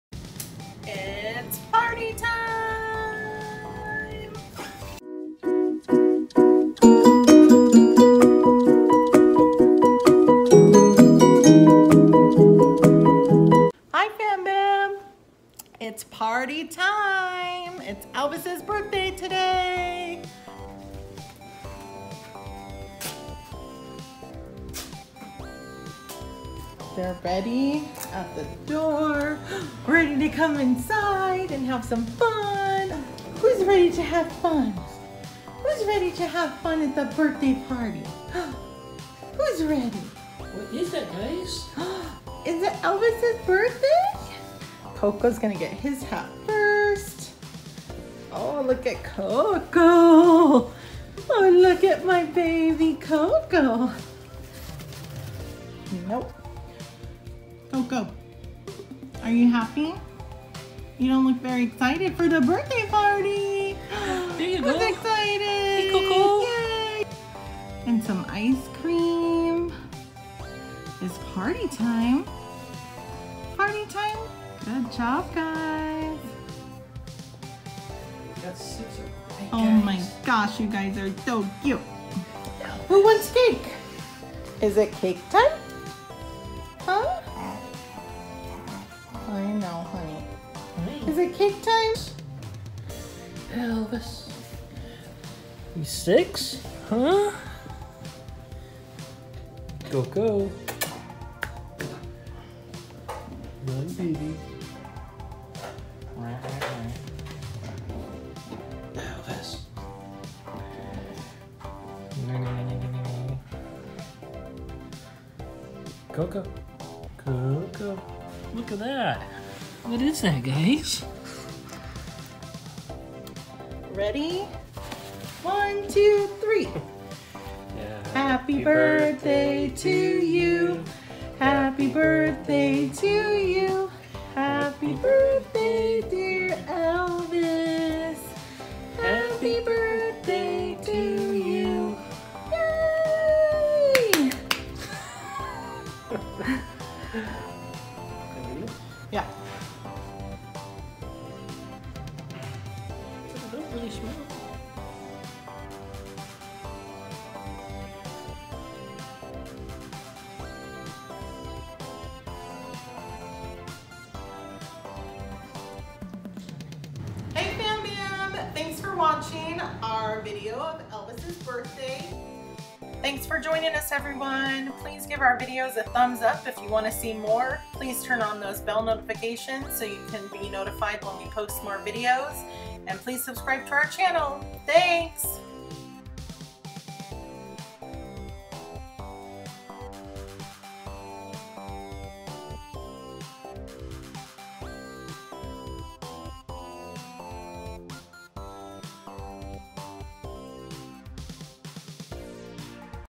PUPPY BIRTHDAY PARTY!!!! - A SHIH TZU NAMED ELVIS
Its Elvis's 6th Birthday Everyone!!!

If you like this video please give us a thumbs up or leave us a Comment to let us know what you think!